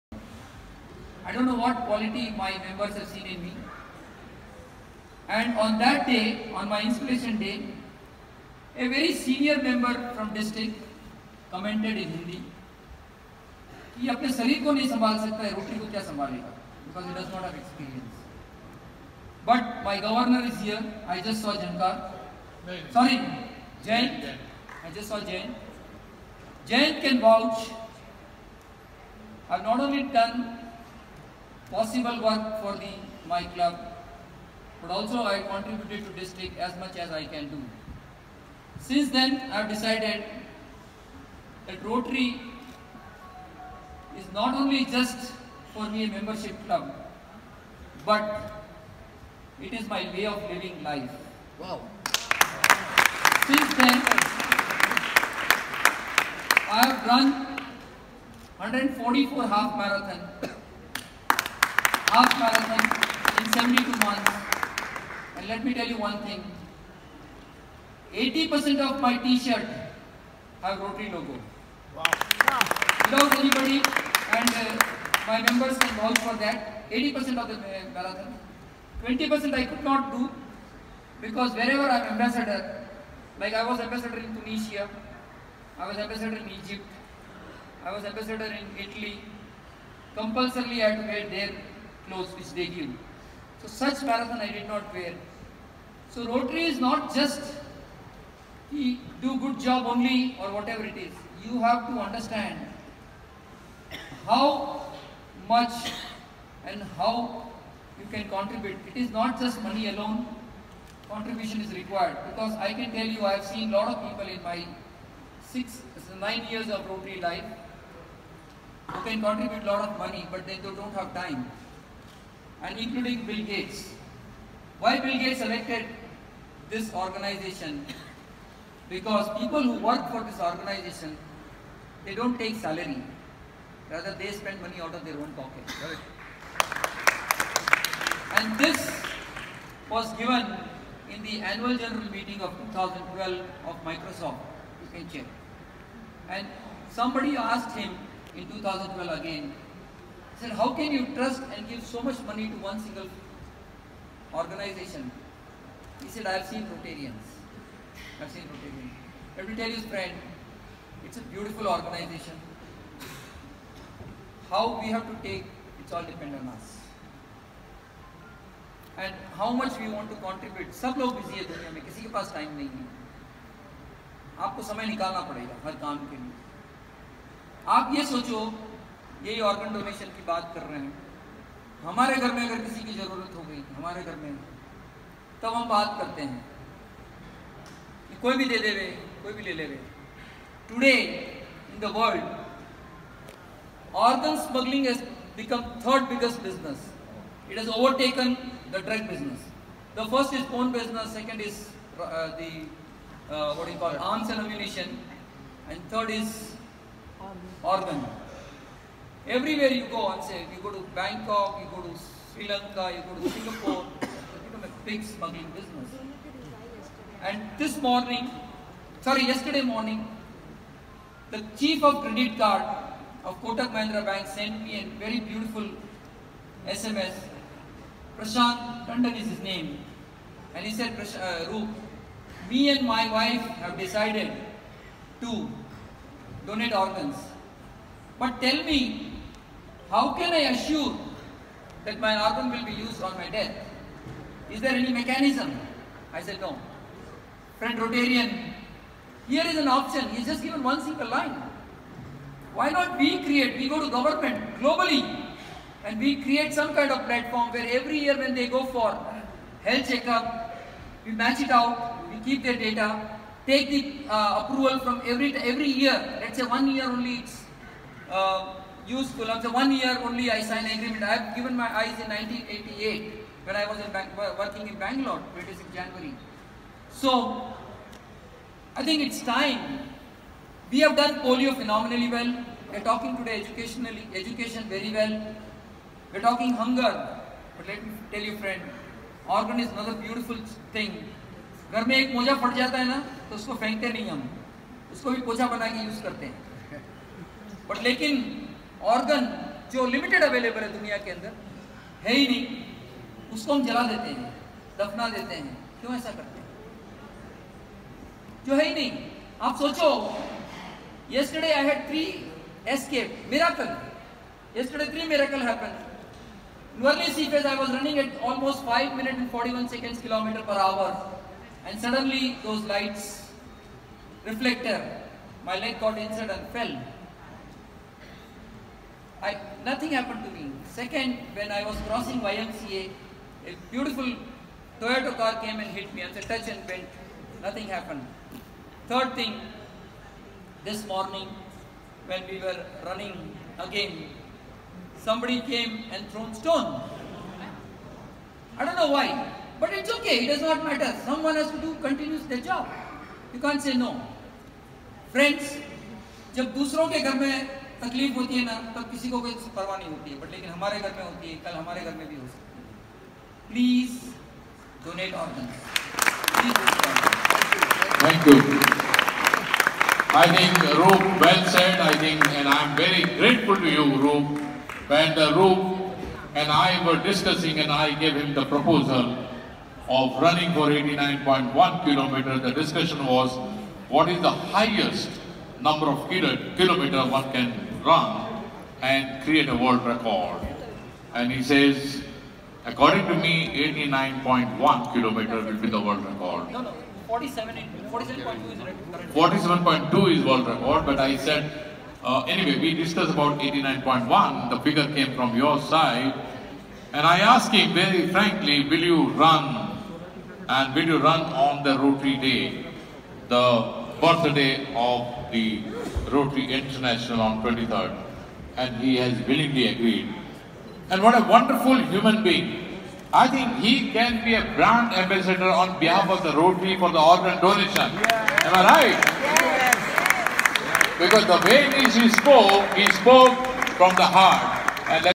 Rtn. Roop Betala - 89.1 Kms Blind Folded Run Guinness Records
A Moment for Life Time. A grand entry into Guinness Book of World Records - running blind folded for 89.1 Kms in 17 hours. As a fellow Rotarian, I am mighty proud and delighted this moment. Your love for Rotary is unparalleled. We know you are doing this to raise funds for Organ Donation. Every word you spoke is priceless.

Congratulations  Roop.

You completed 6 glorious years of running (23rd Feb 2013- 23rd February 2019). In the last 72 months you completed (competitive) 144 Half Marathons in 41 countries and 113 cities.

We are proud that with you the Indian flag has traveled 41 countries and in each country it has traveled a minimum of 21.1KM with pride and respect.